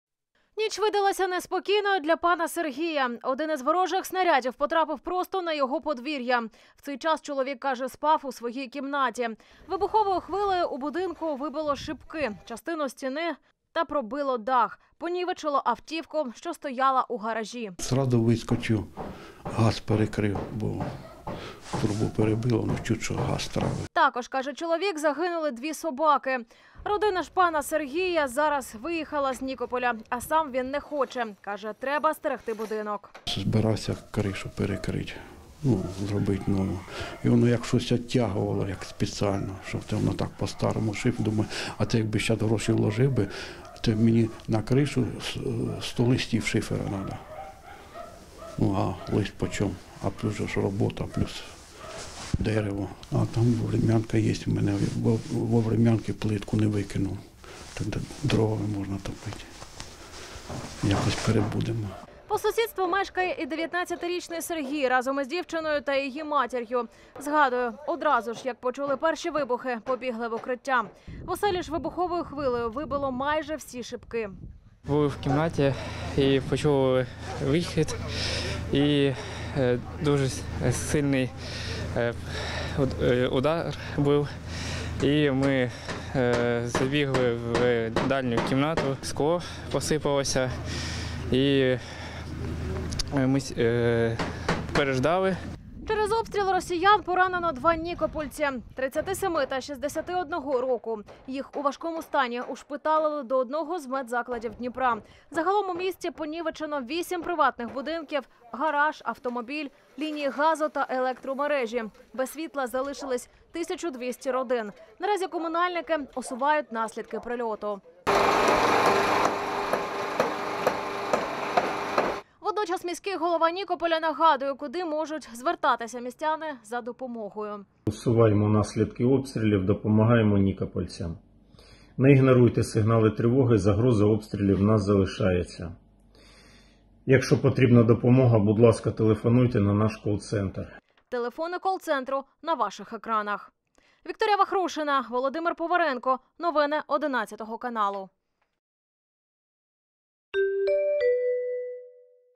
🤯Двоє ПОРАНЕНИХ. Наслідки УДАРУ по Нікополю
Вибуховою хвилею у будинку вибило шибки, частину стіни та пробило дах. Понівичело автівку, що стояла у гаражі.